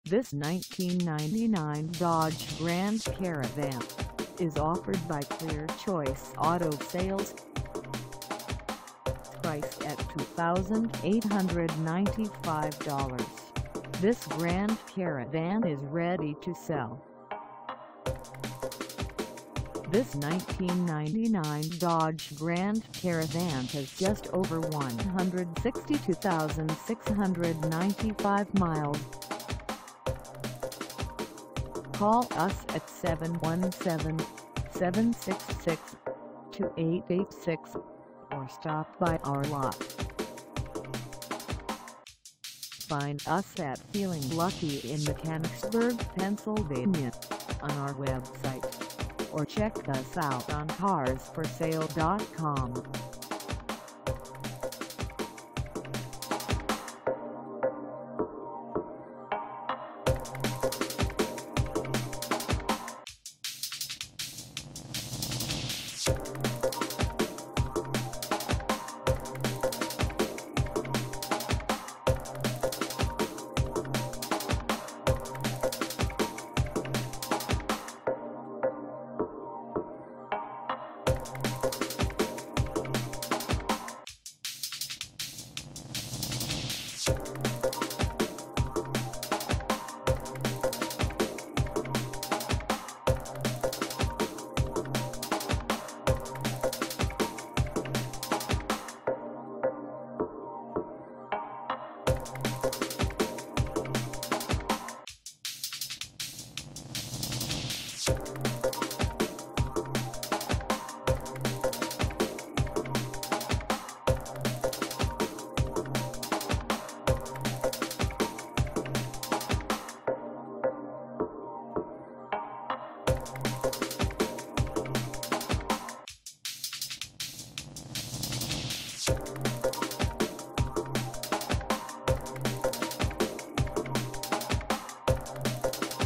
1999 Dodge Grand Caravan SE for sale in Mechanicsburg, PA 17
This 1999 Dodge Grand Caravan SE is for sale in Mechanicsburg, PA 17050 at Clear Choice Auto Sales.
Engine: 6-Cylinder V6, 3.3L
Transmission: 4 Speed Automatic
Color: Blue
Mileage: 162695
Contact Clear Choice Auto Sales 5200 Simpson Ferry Road Mechanicsburg, PA 17050 to test drive this 1999 Dodge Grand Caravan SE today.